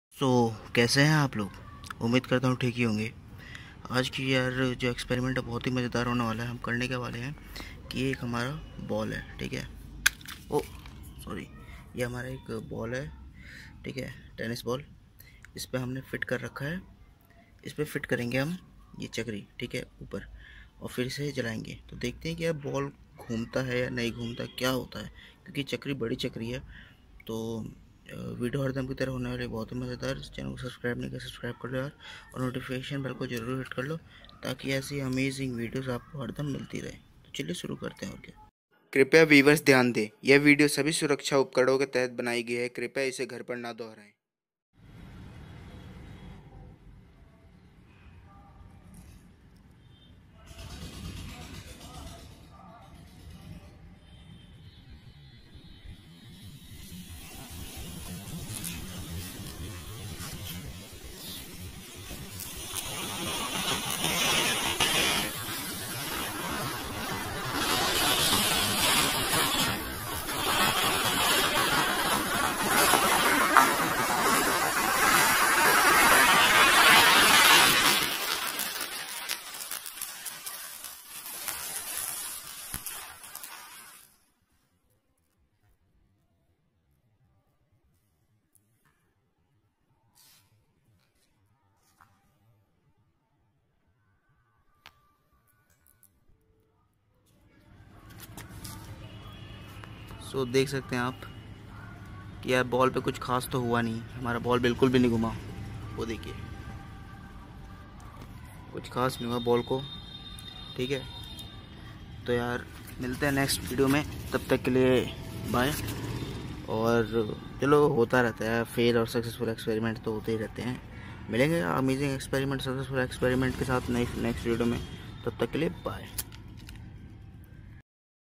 चकरी को लगाया बॉल पर | Experiment : Chakri On Ball | Chakri Vs Tennis Ball | Diwali Experiments
Iss video hum ek chakri ko tennis ball or ek cosco ki ball ke upar lagayenge or test karenge ki kya hota hai.

Chakri Powered Ball...